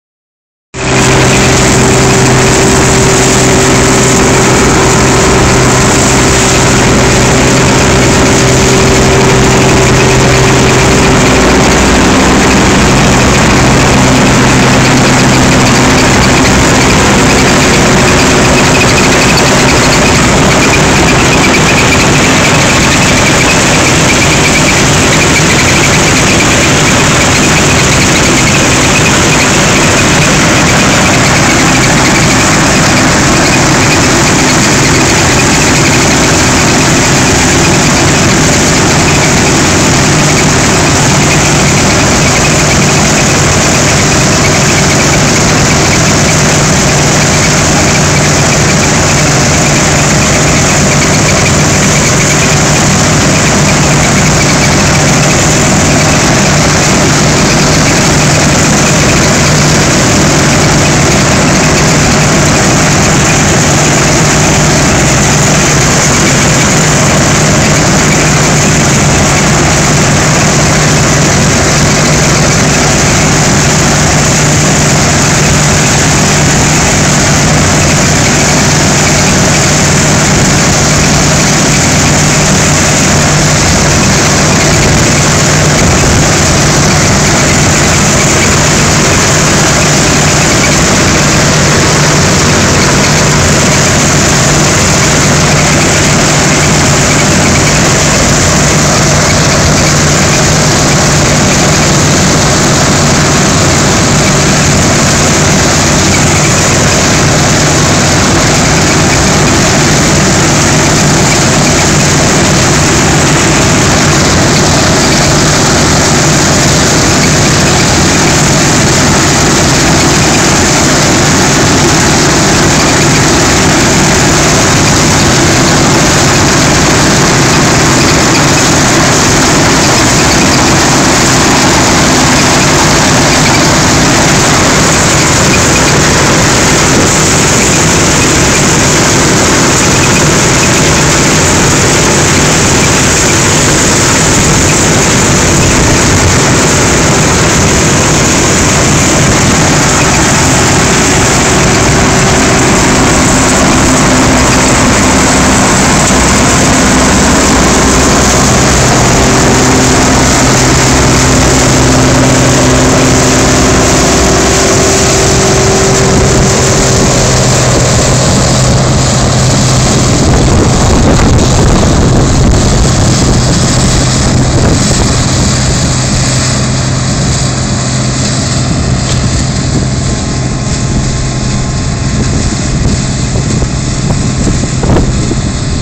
raps dreschen in krautheim 3
Claas Diminator 98 VX mit Rapsschneidwerk C450